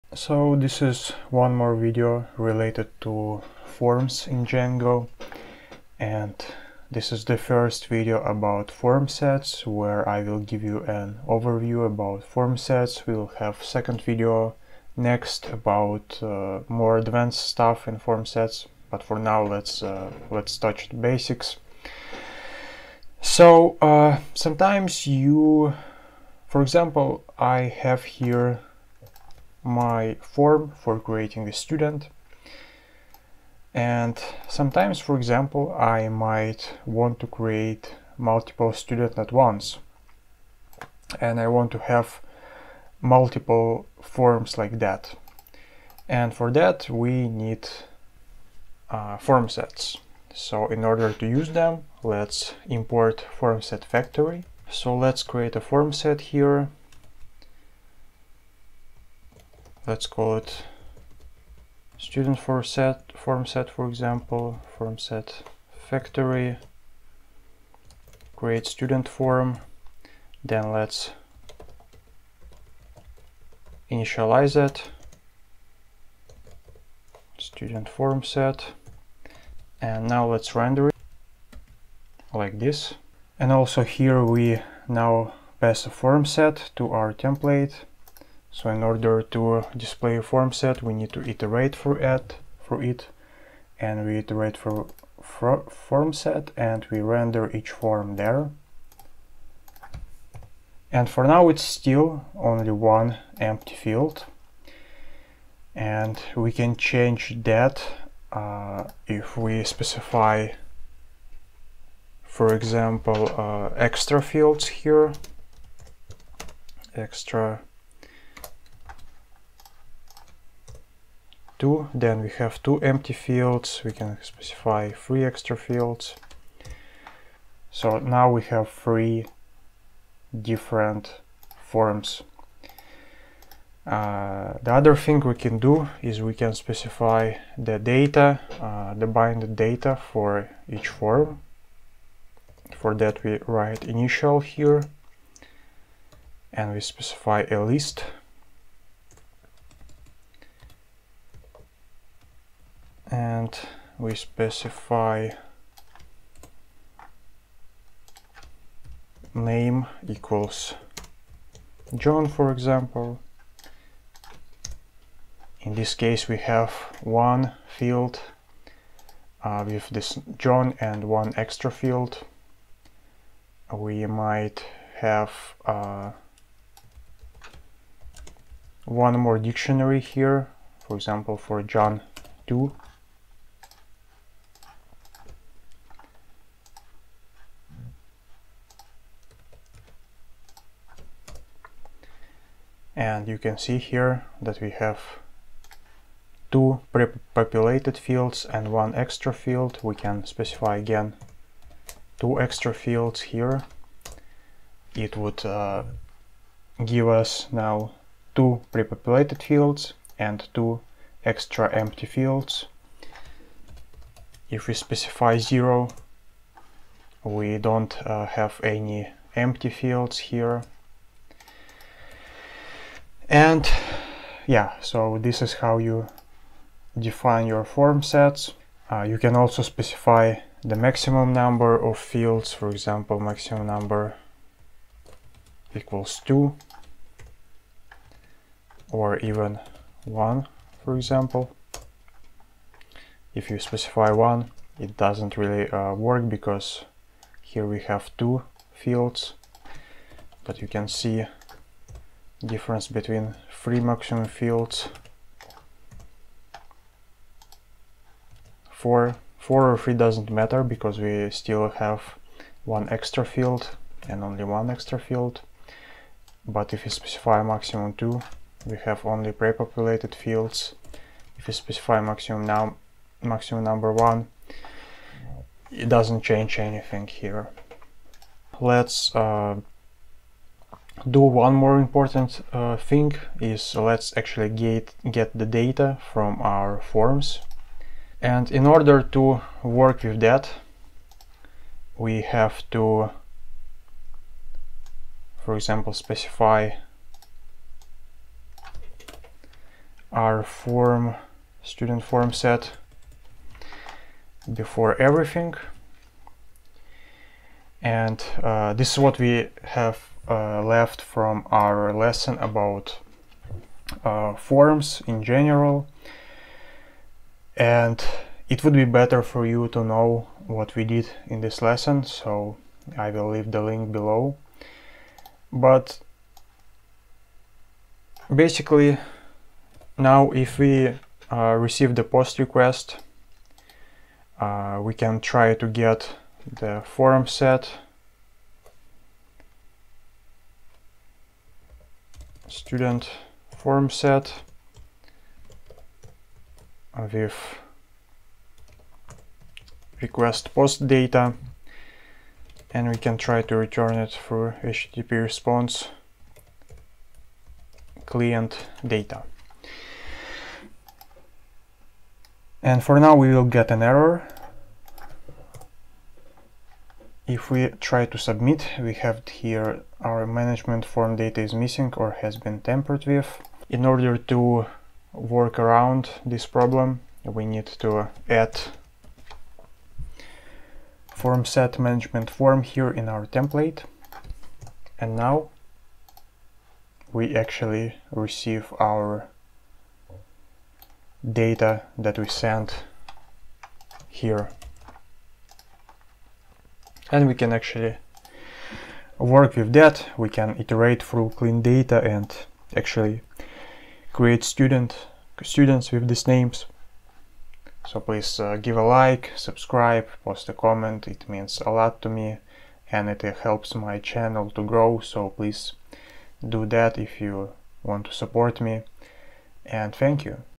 Django Full Course - 16.0 - Django Formsets. Defining, displaying, getting data from formset
In this video from Django Full Course we will work with Django Formsets:
- How to define Django formsets,
- How to display formsets in template,
- How to get data from formset.